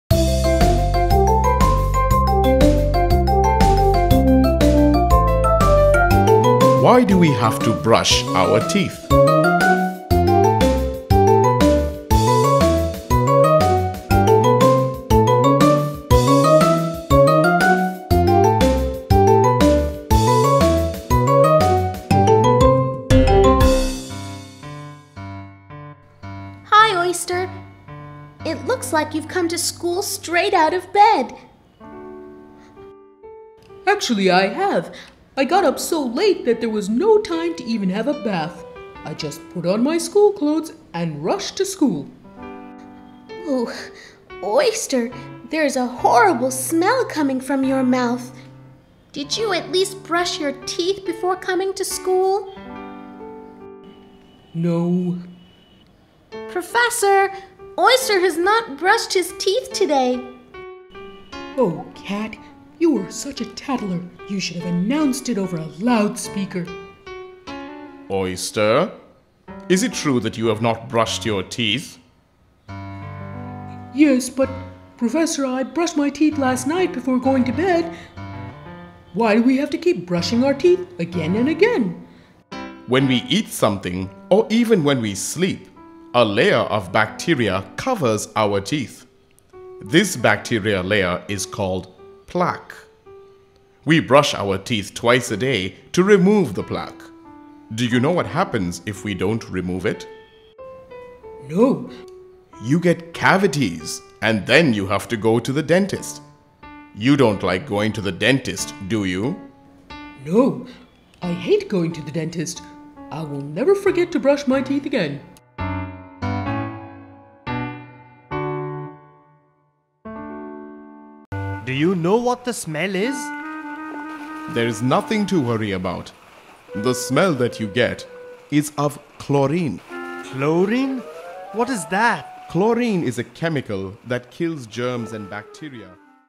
Why Do We Have To Brush Our Teeth? | Kids Health Tips | Interesting Facts For Kids
Interesting facts about kids brushing teeth. Why do We have to Brush Our Teeth?Kids Health Tips. How to have healthy teeth for kids? Kids ask such simple yet tricky questions at times. To help you answer them, we are right here with this HD video. Hey kids, have fun and also learn through the cute 3D animation. Brushing our teeth regularly helps to remove food and plaque, which is a sticky film that forms on your teeth and contains bacteria. Watch the video to know more.